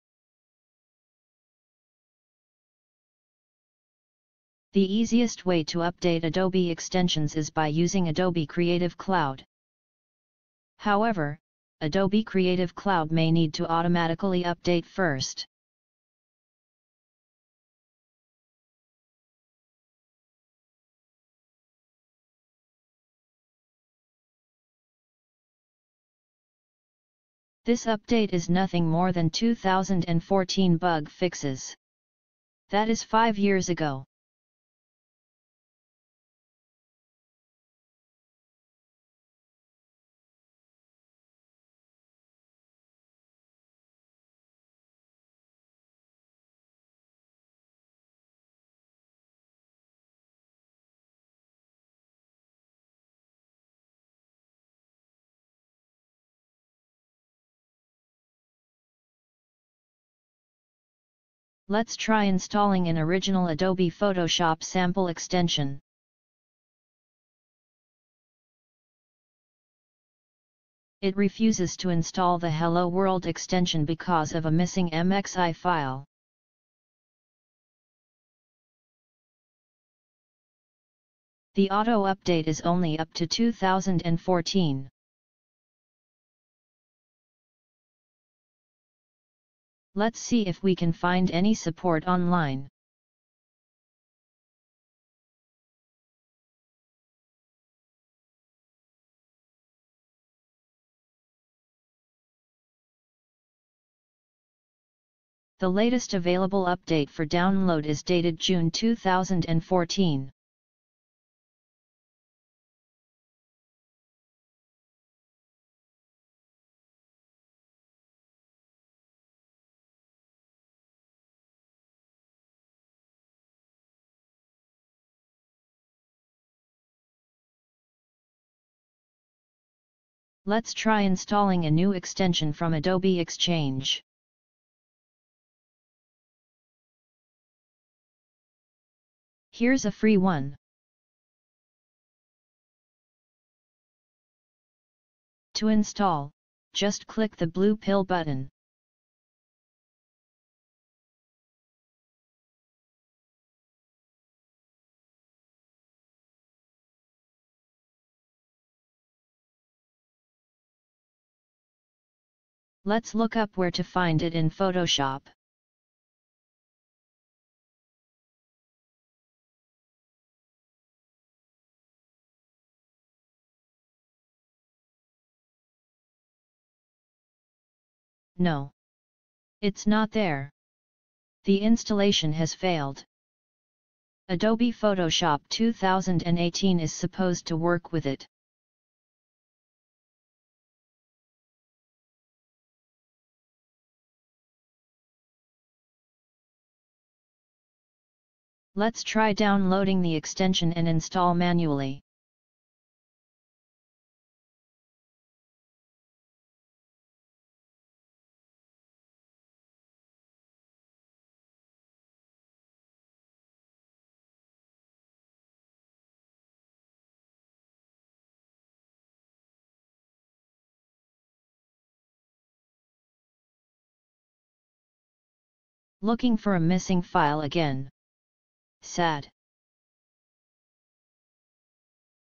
Signs That Adobe Extensions Manager May Be Abandoned
Priority on the Adobe Extensions Manager, last updated in 2014, may have gone next to total abandonment by Adobe engineers. After all, this is just support for Adobe Creative Suite extensions which has little impact on main program sales. Extension users are currently left scratching their heads on how to get new extensions, whether from the Adobe Exchange or other download sources, to actually install and work within any of the Adobe CC programs.